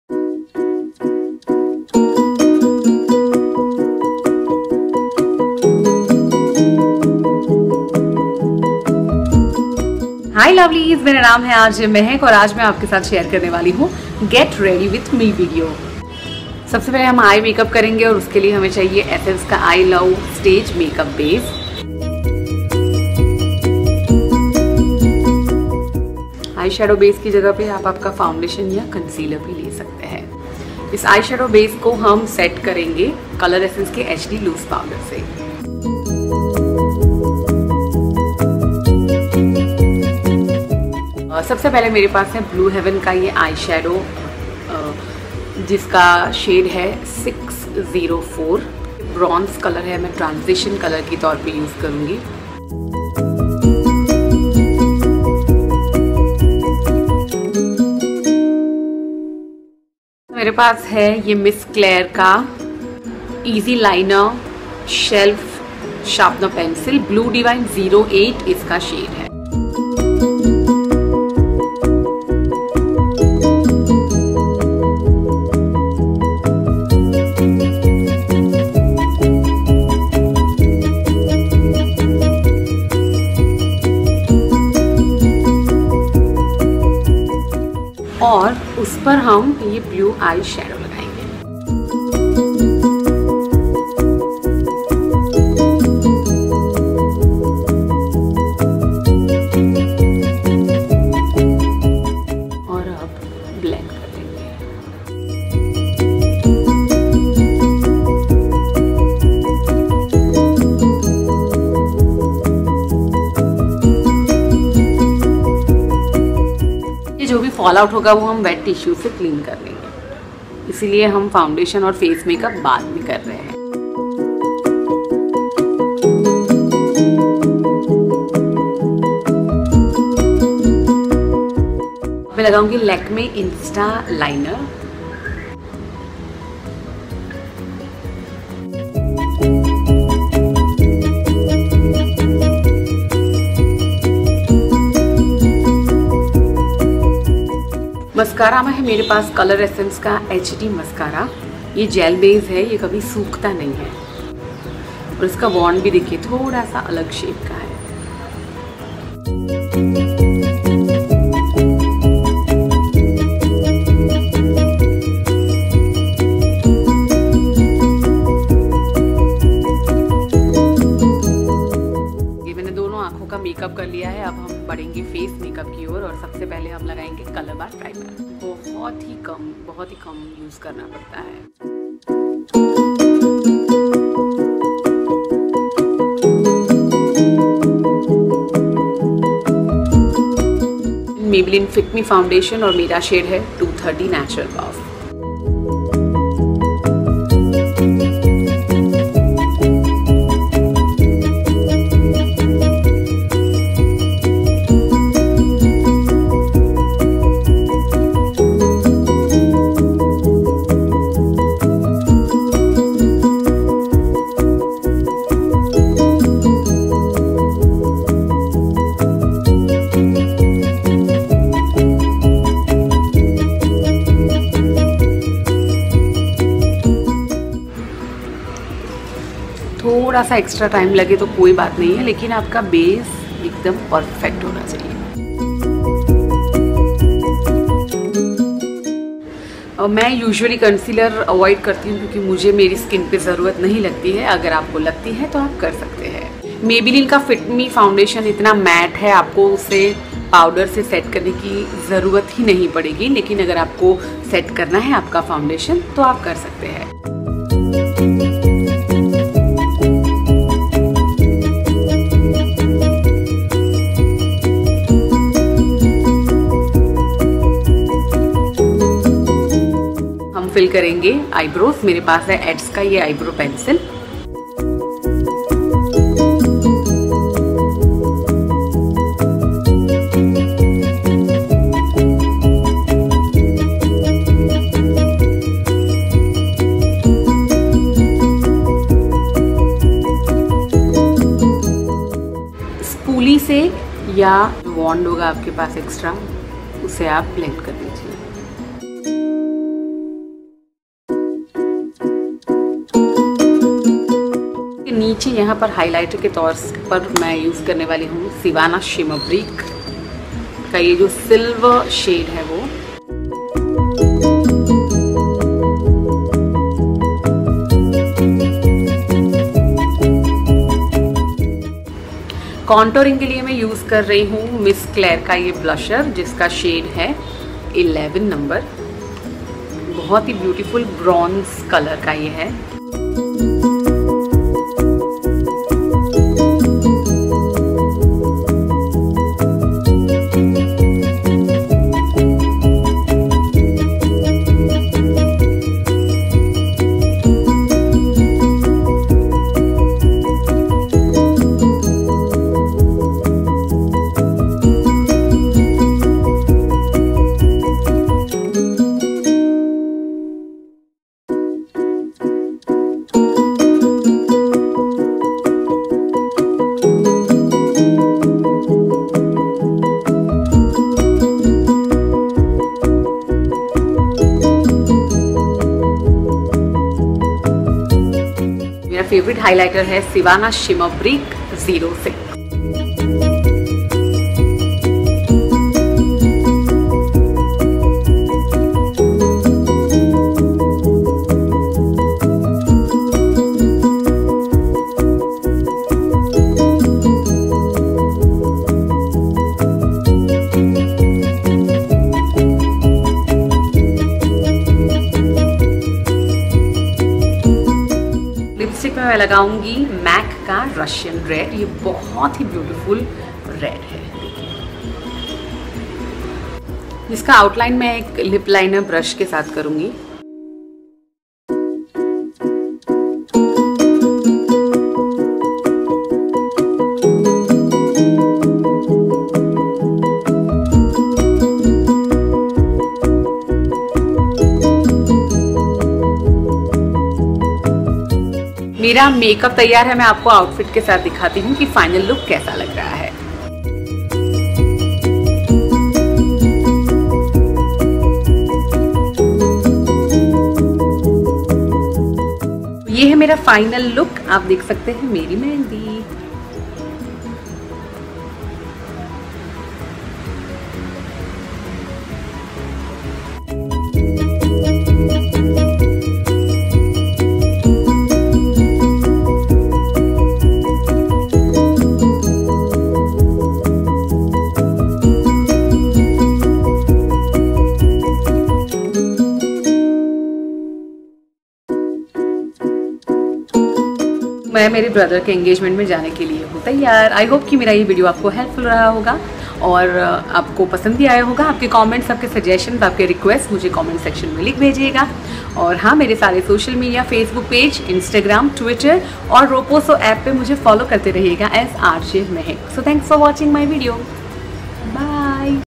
Makeup Tutorial | Blue Eye Shadow | Red lips | Affordable Makeup
here is a Get Ready With Me Video

Products used
1 Essence i love stage eyeshadow base
2 coloressence hd loose powder - FP3
3. Miss Claire eye pencil - blue Devine
4. Blue heaven eye shadow -  604
5  lakme insta liner
6 maybelline colossal kajal
7 coloressence hd mascara
8 colorbar primer
9 maybelline fit me foundation - 230
10 miss Claire blusher - 011
11 sivanna shimmer brick - 06
12 ads eye brow pencil
13 MAC Russian  Red lipstick